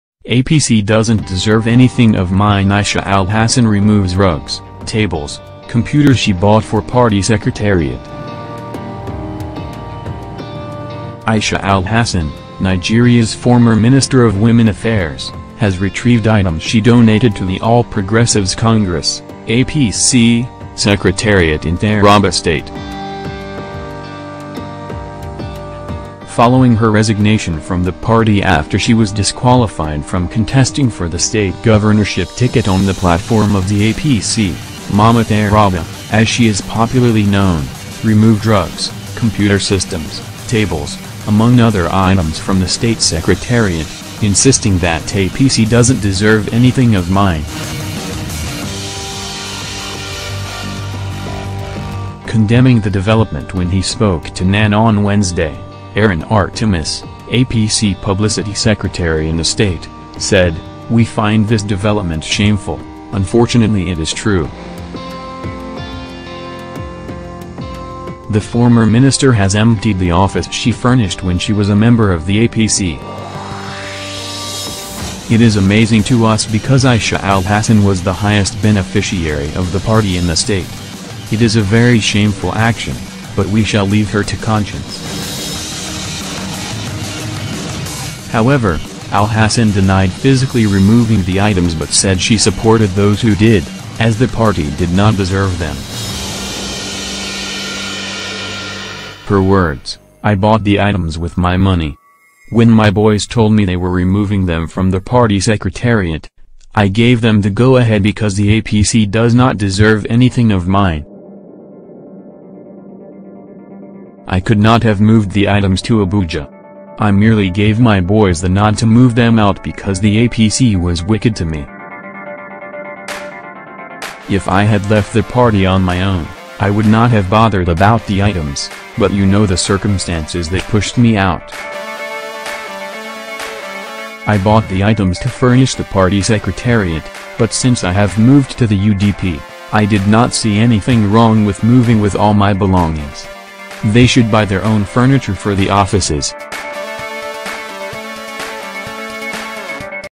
Aisha Alhassan Removes Rugs, Tables, Computers She Bought For Party Secretariat
“I could not have moved the items to Abuja. I merely gave my boys the nod to move them out because the APC was wicked to me. If I had left the party on my own, I would not have bothered about the items, but you know the circumstances that pushed me out," she said. “I bought the items to furnish the party secretariat, but since I have moved to the UDP, I did not see anything wrong with moving with all my belongings. They should buy their own furniture for the offices."